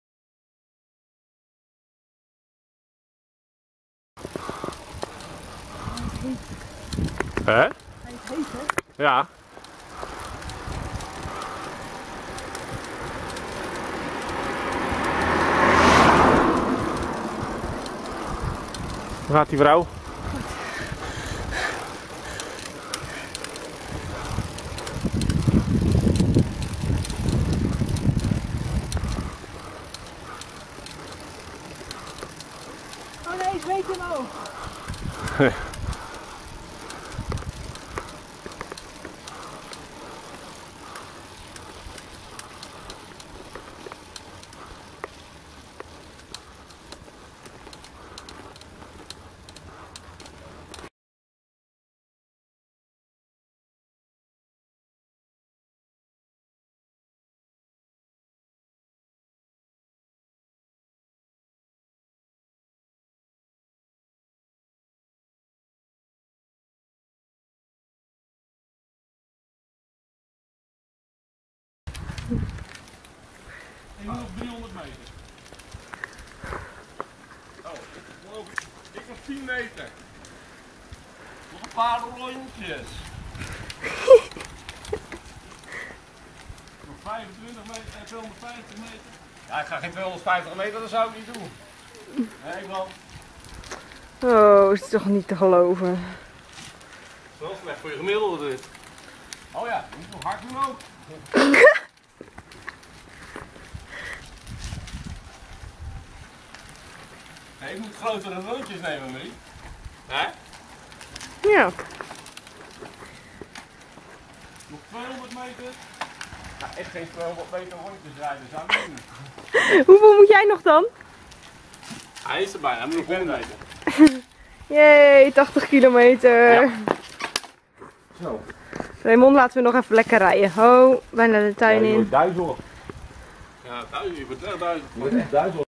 Spanje zomer 2014 - Beklimming Vall d' Ebo en nog wat onzin
De super mooie Vall d' Ebo beklommen en toen we bij thuiskomst net geen 80km hadden gereden hadden de mannen daar iets op bedacht...